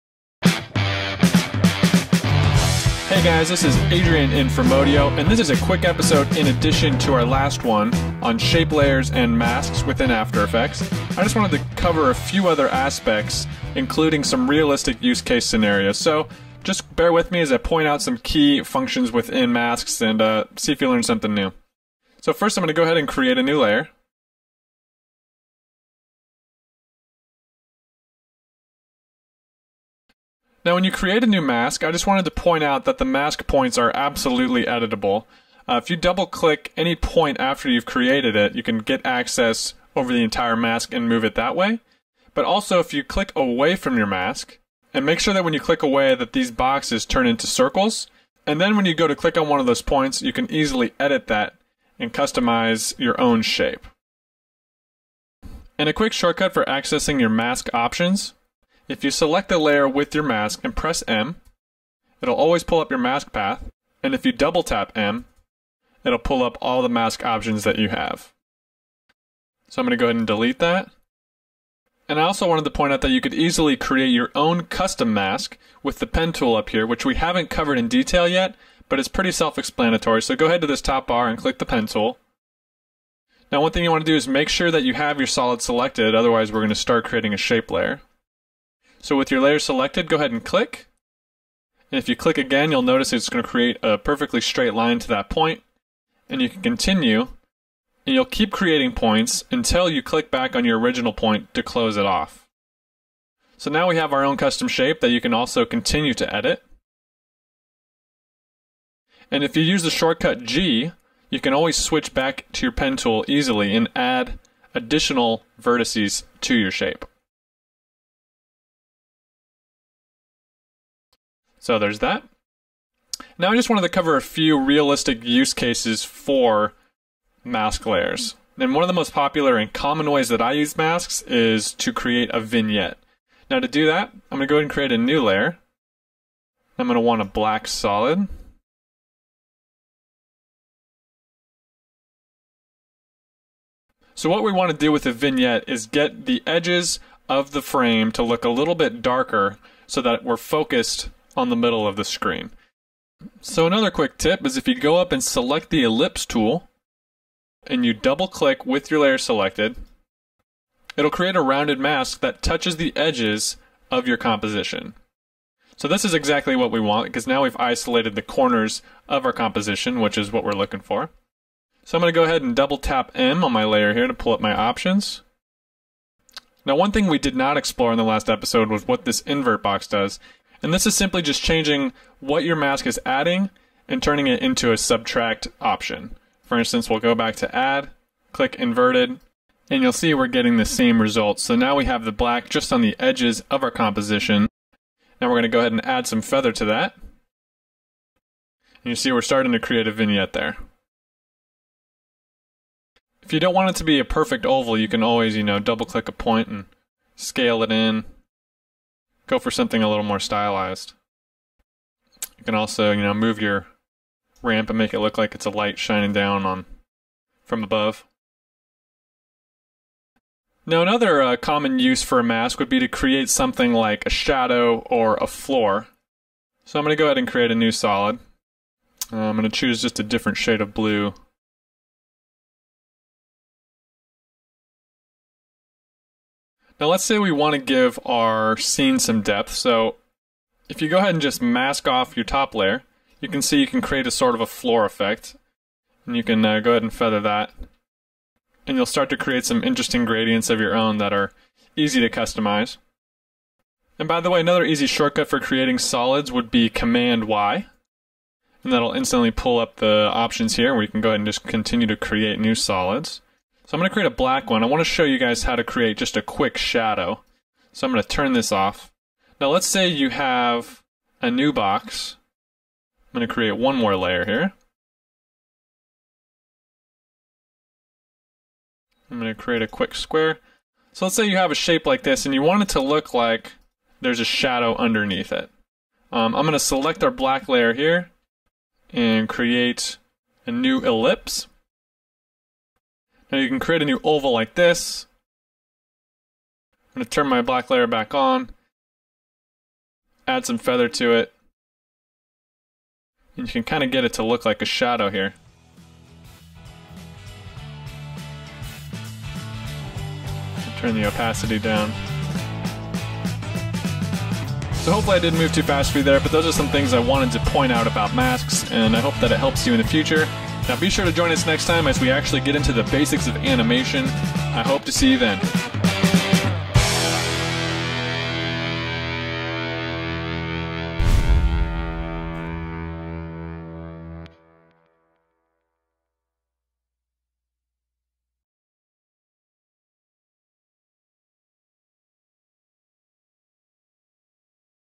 After Effects Basics #4 - Using Masks & How to Create a Vignette
Topics Covered:
• How to Create a Custom Mask
• How to Edit a Custom Mask
• How to Create a Vignette
• Mask Tool Shortcuts
• Create a New Solid Shortcut (Mac: Command+Y - Windows: Ctrl+Y)